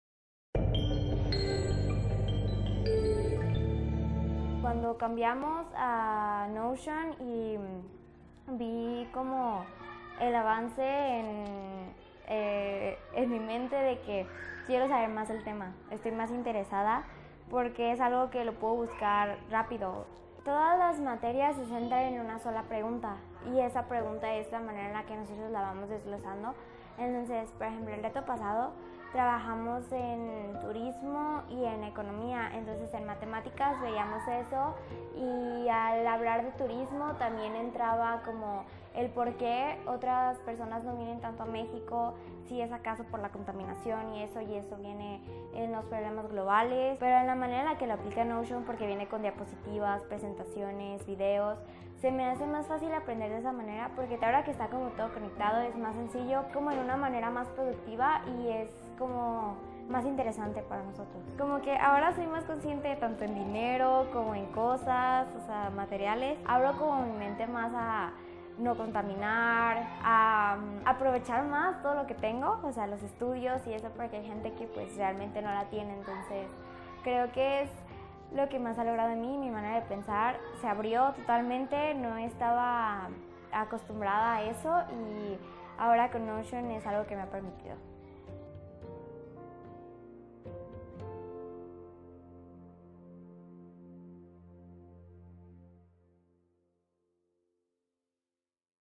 Ismene Beas - Alumna del Colegio IMI l Knotion para Alumnos
Ella es Ismene Beas, alumna del Colegio IMI.
Aprender de esta manera es más fácil porque todo está conectado, es más productivo e interesante para nosotros.